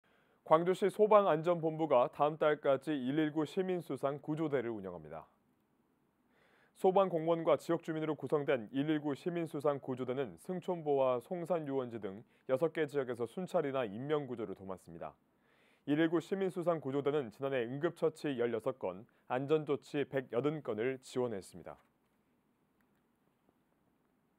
[광주뉴스] 광주시소방안전본부, 8월까지 119시민수상구조대 운영
CMB 광주방송